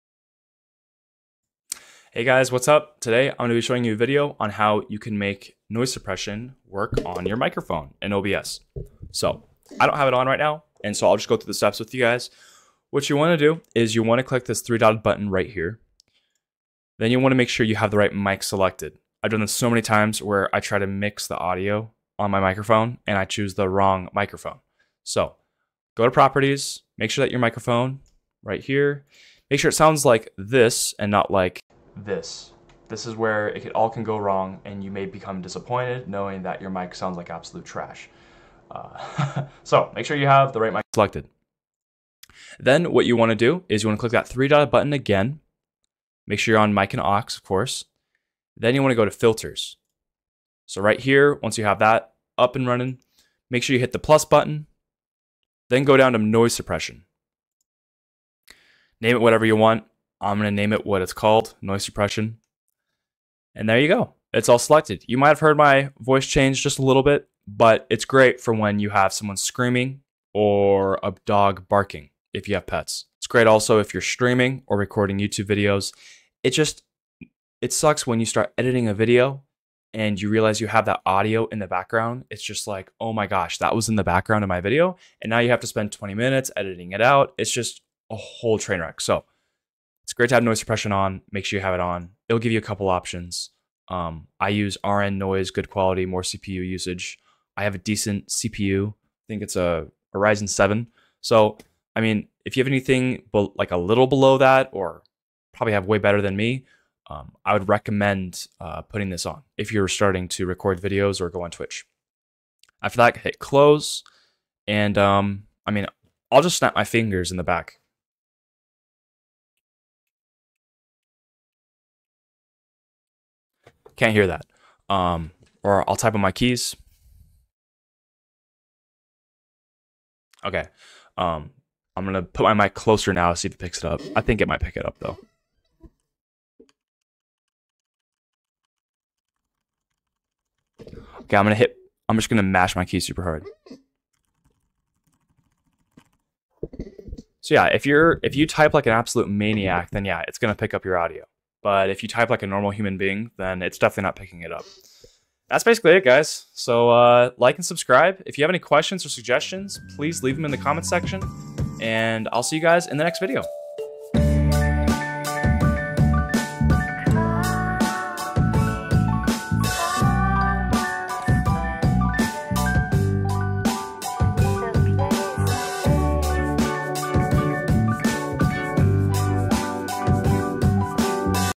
How to Add Noise Suppression to Audio Inputs using OBS
OBS (Open Broadcast Studio) is packed with features. Did you know you can use OBS's built-in feature to suppress background noise from your microphone? It's the default setting for all my videos, so I figured it would be a great tip to demo and show you how to configure it.